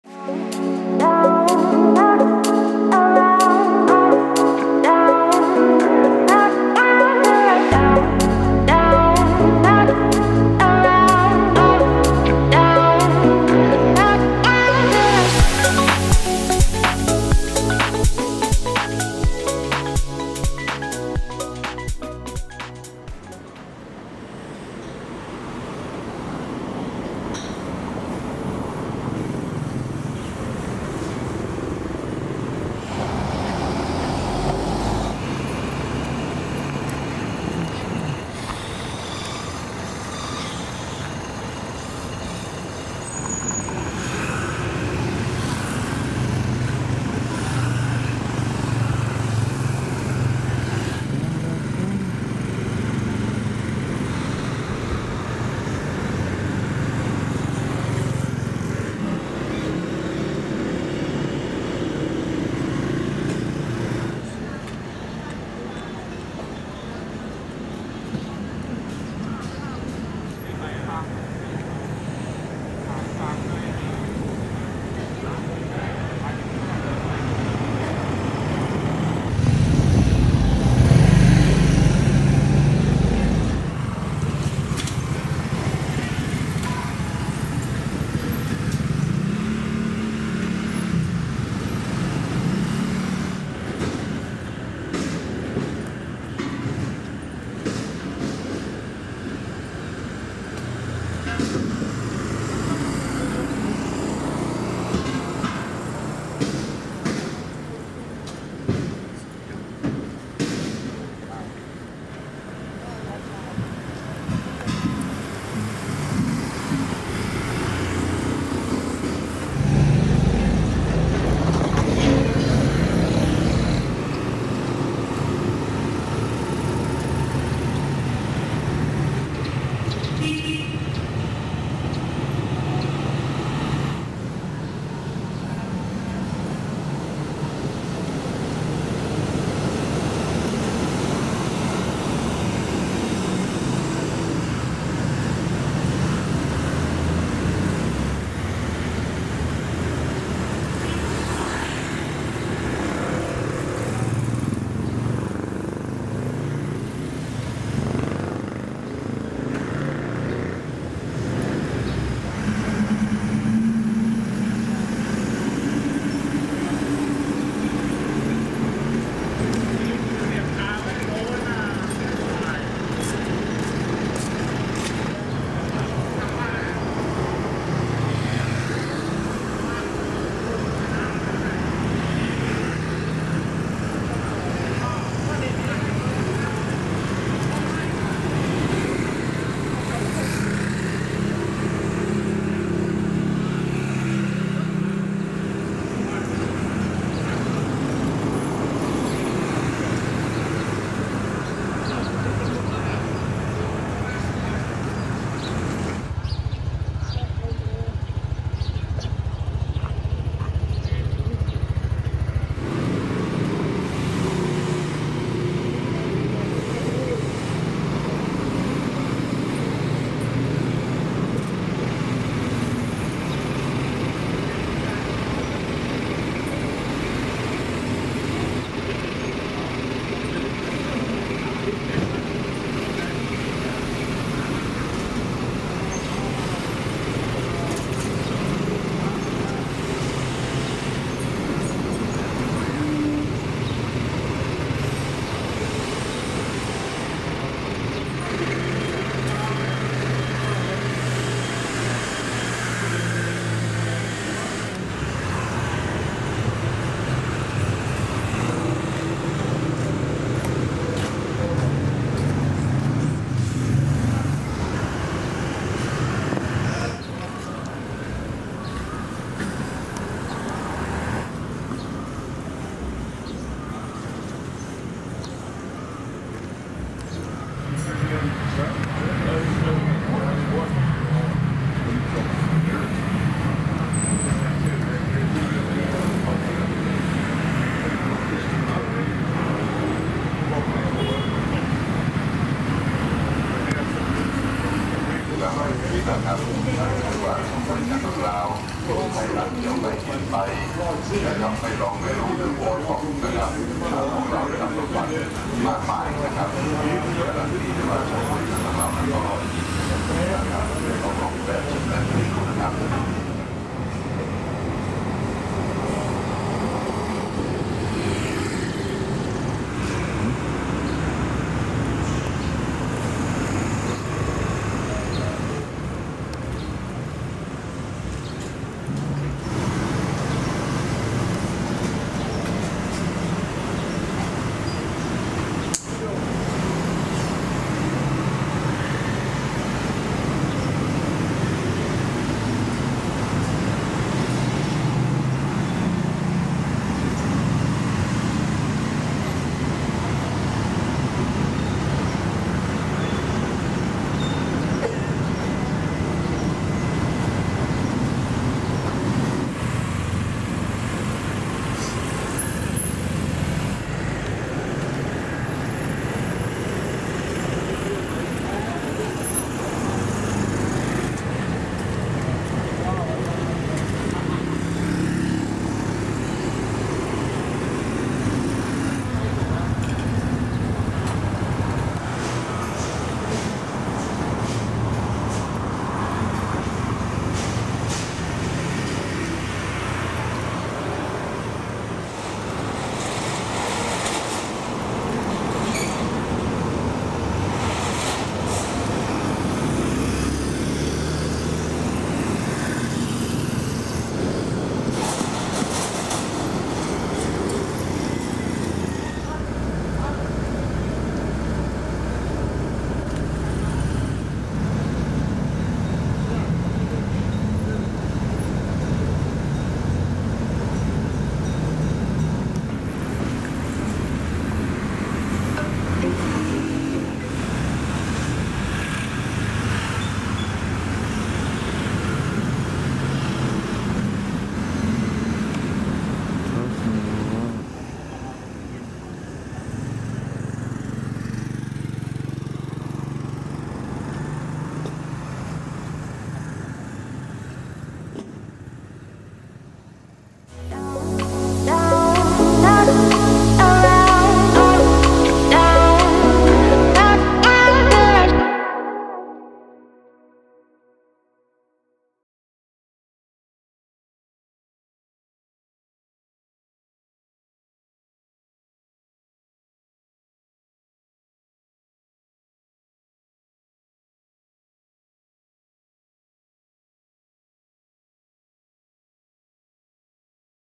OMG SHE IS PERFECTION ITSELF, Pattaya Soi Buakhao, Thailand 2023
Thailand Vlogs. Pattaya South Road, Soi Buakhao Beautiful Daytime Scenes.